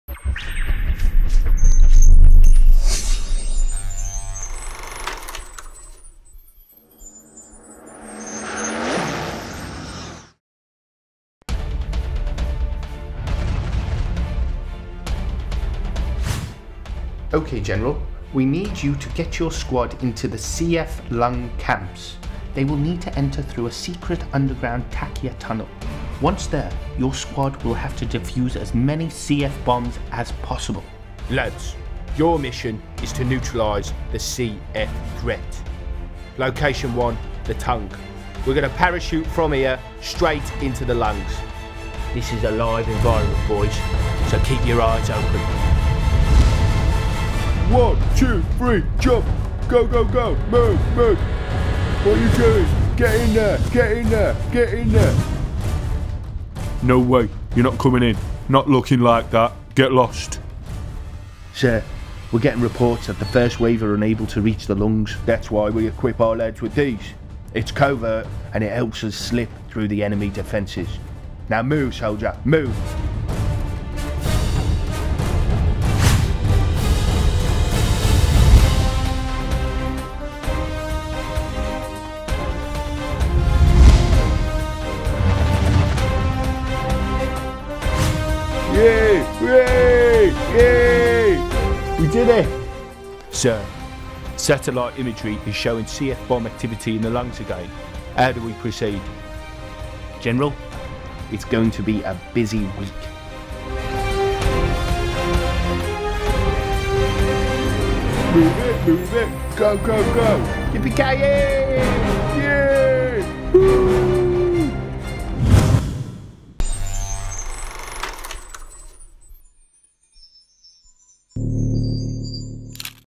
What is Gene Therapy - Animation
This clever little animation explains the process of gene therapy for Cystic Fibrosis ... and it was designed by Changing Futures participant Caitlin. This film is part of the Changing Futures Project